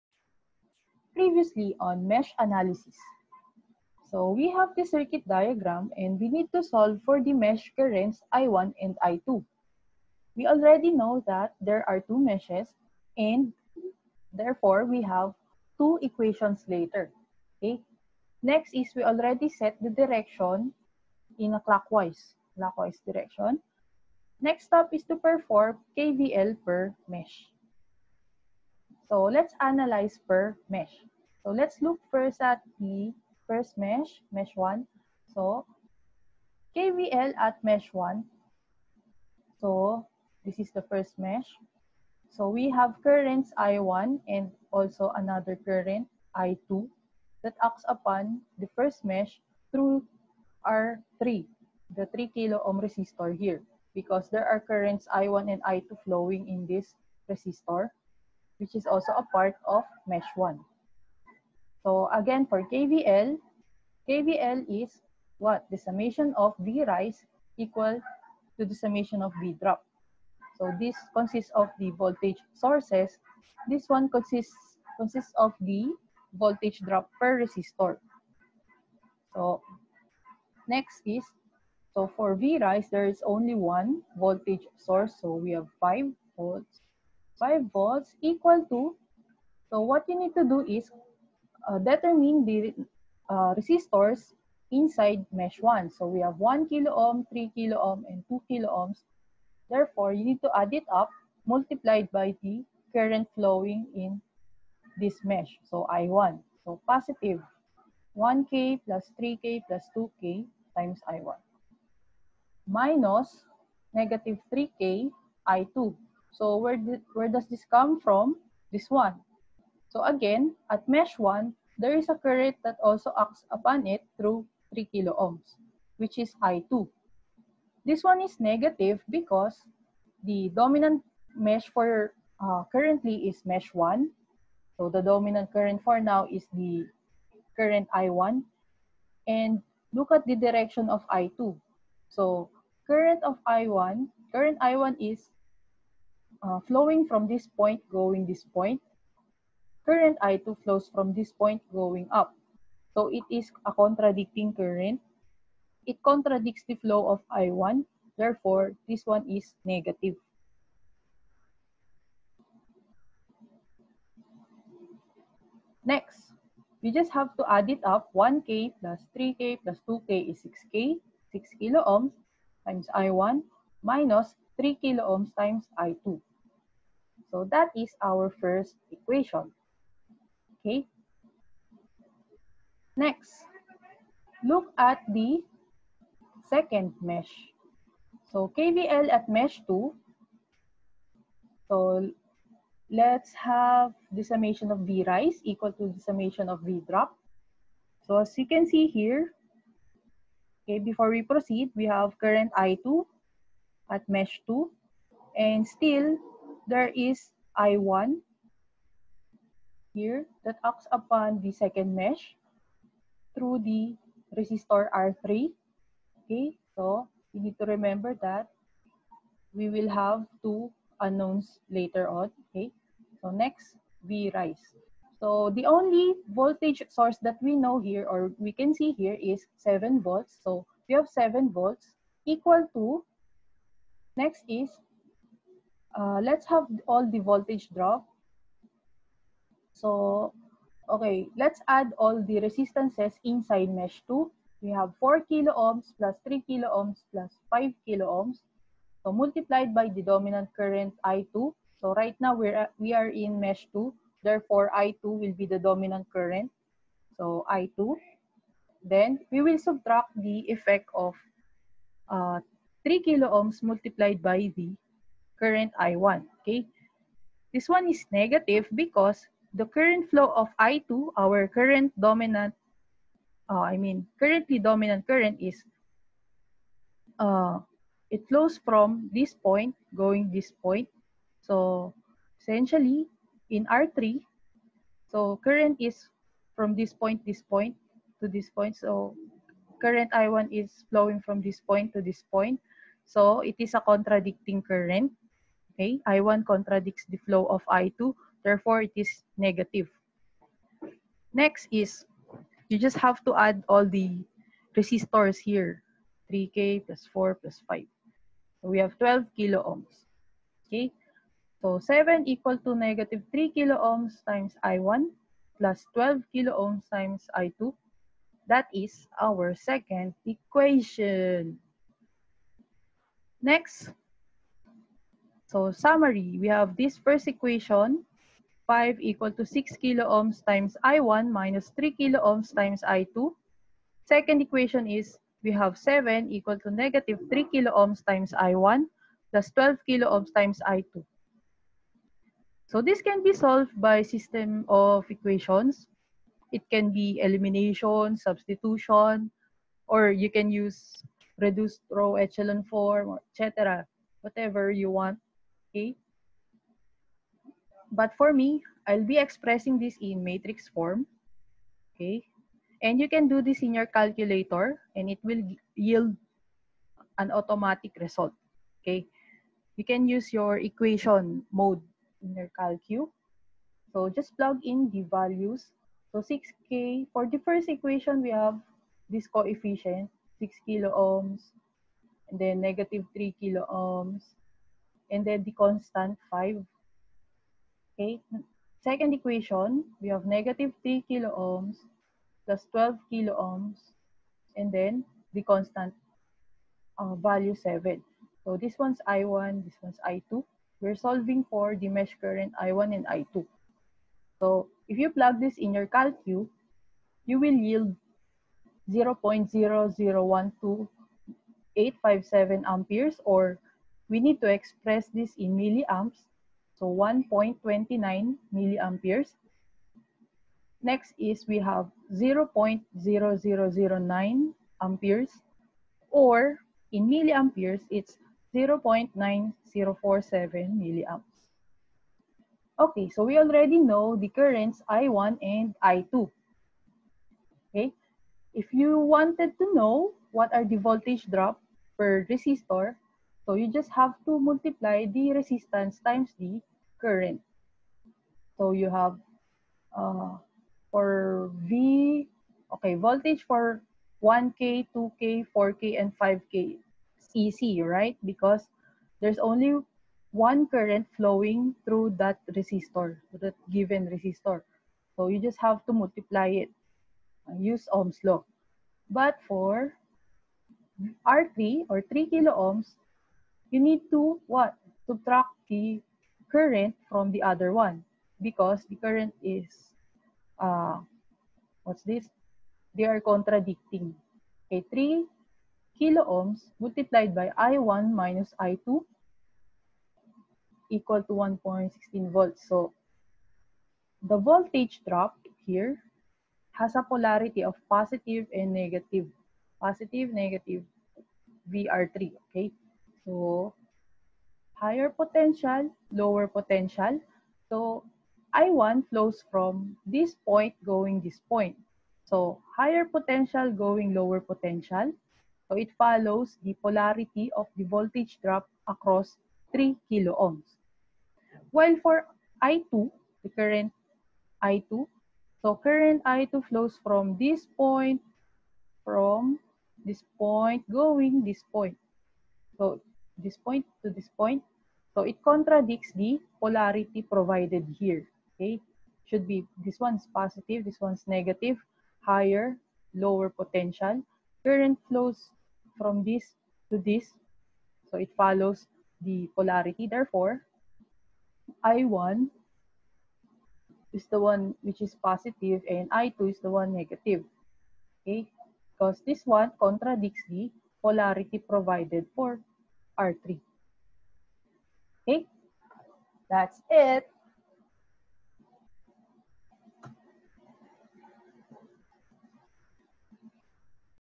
How to use Mesh Analysis ? Part 2
Provided example to demonstrate Mesh analysis involving independent sources and voltage sources only.